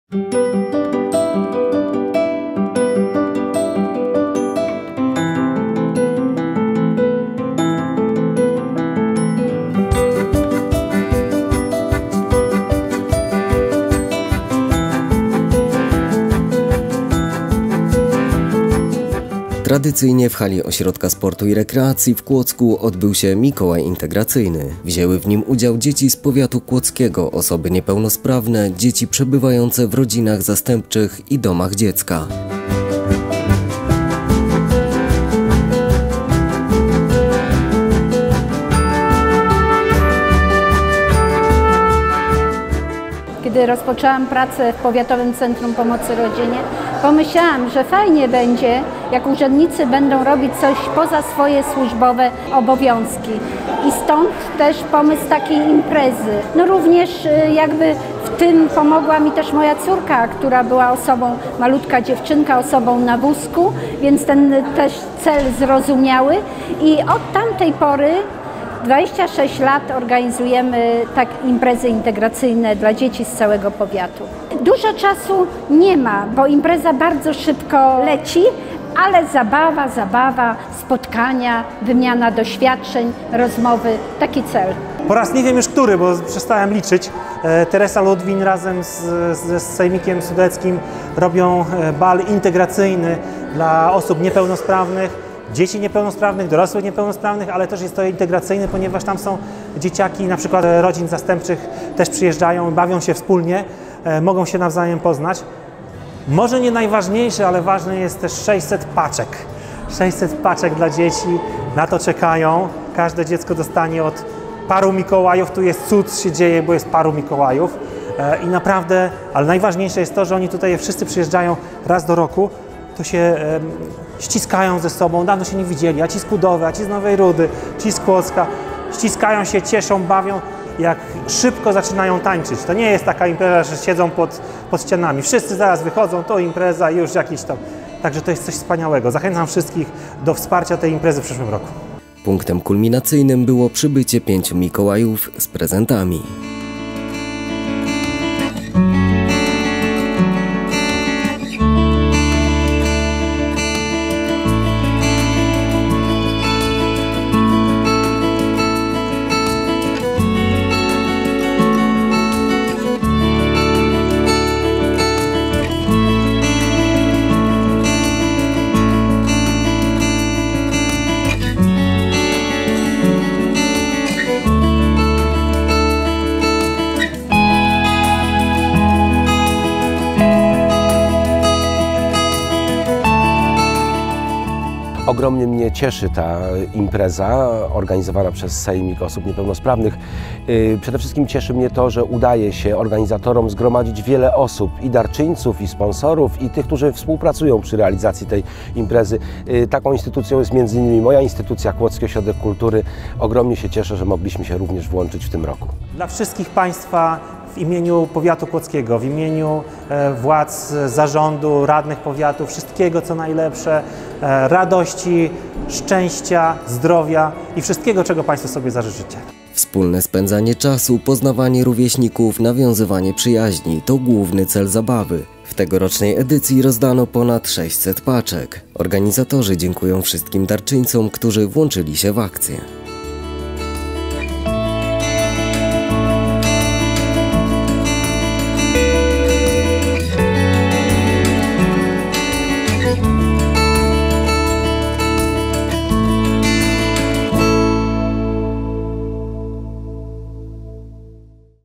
MIKOŁAJ INTEGRACYJNY W KŁODZKU
W hali Ośrodka Sportu i Rekreacji w Kłodzku odbył się Mikołaj Integracyjny. Wzięły w nim udział dzieci z powiatu kłodzkiego, osoby niepełnosprawne, dzieci przebywające w rodzinach zastępczych i domach dziecka. Punktem kulminacyjnym było przybycie pięciu Mikołajów z prezentami dla przybyłych gości.
Wspólne spędzanie czasu, poznawanie rówieśników, nawiązywanie przyjaźni – to główny cel zabawy. W tegorocznej edycji rozdano ponad 600 paczek. Organizatorzy dziękują wszystkim darczyńcom, którzy włączyli się w akcję.